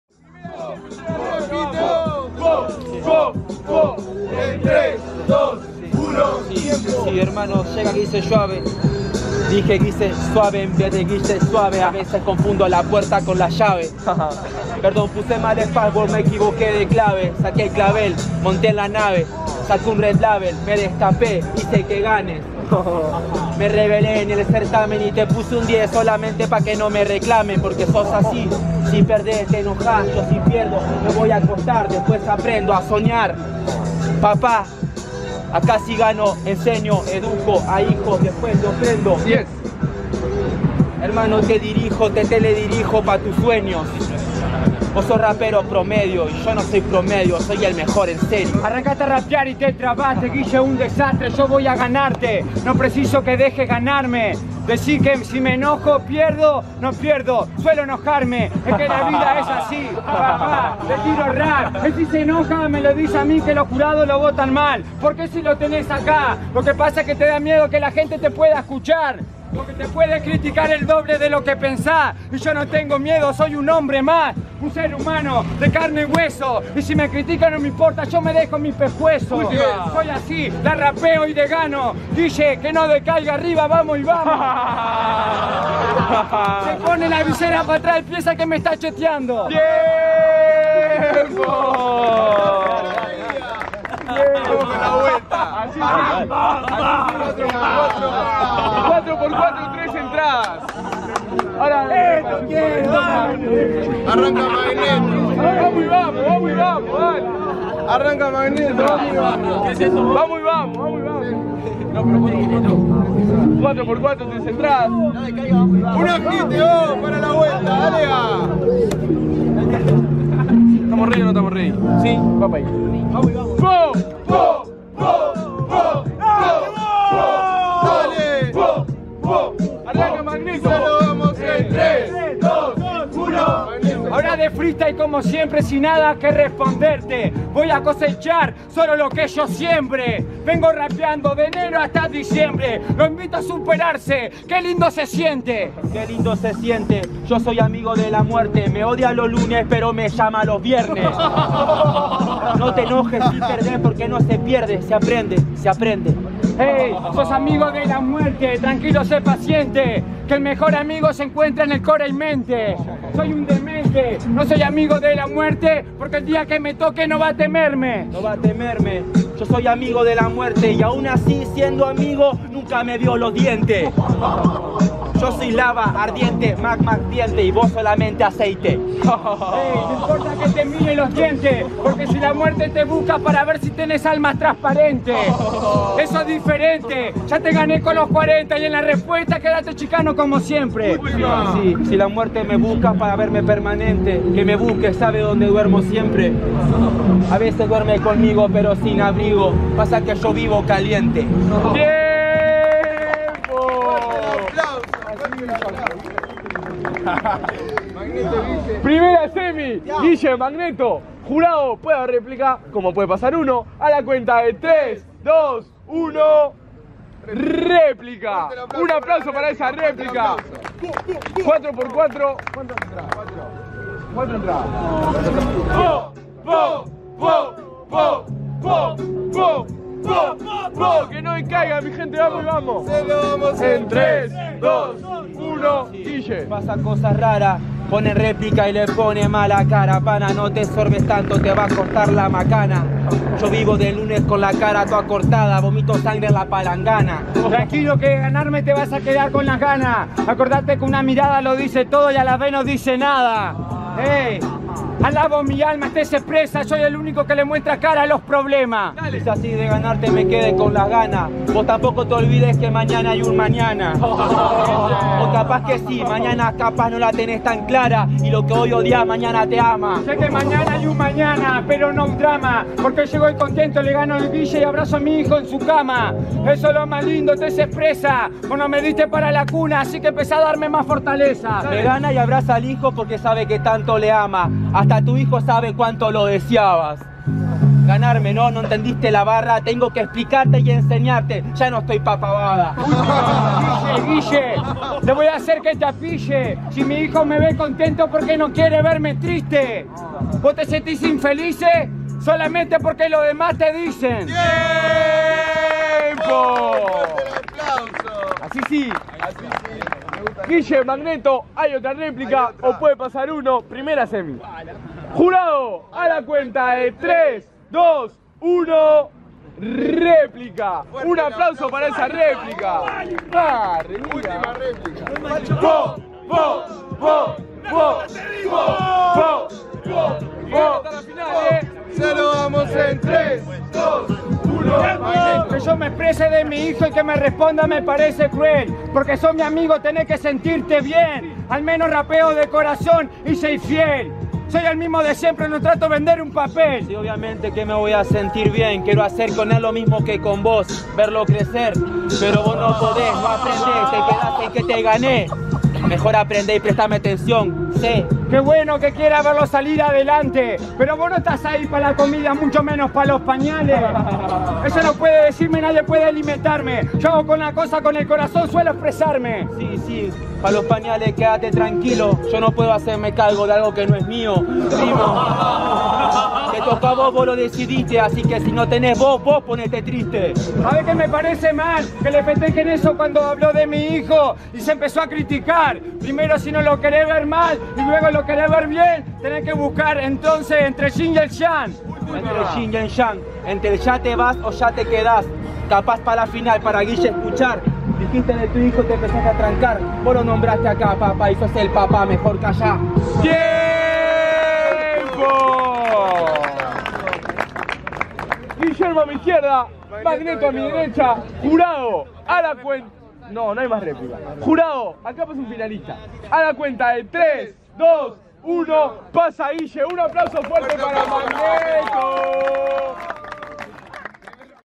GUILLE vs MAGNETO | SEMIFINAL | Fecha 3 | (Torneo de Temporada 2023)
Competencia de Freestyle realizada en la plaza primero de Mayo - Uruguay
Tercera fecha del torneo de temporada con regional a Ciudad del Punch